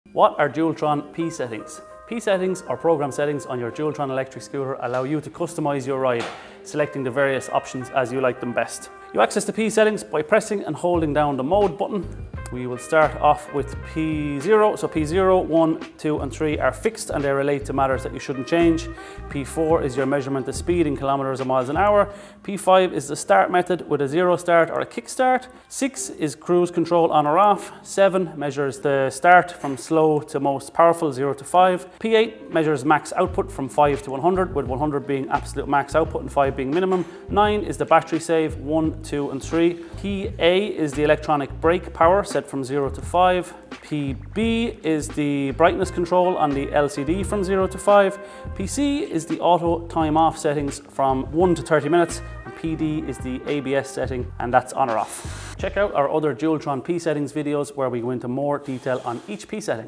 Dualtron Electric Scooters: P-Settings Explained 🛴⚡
P-settings on your Dualtron electric scooter allow you to customise your ride by selecting various options as you like them best.

From speed measurement to battery save options and electronic brake power, the P-settings let you tailor your ride experience.

LOCO Scooters is an Irish-owned and Dublin-based electric scooter specialist retailer and repair centre.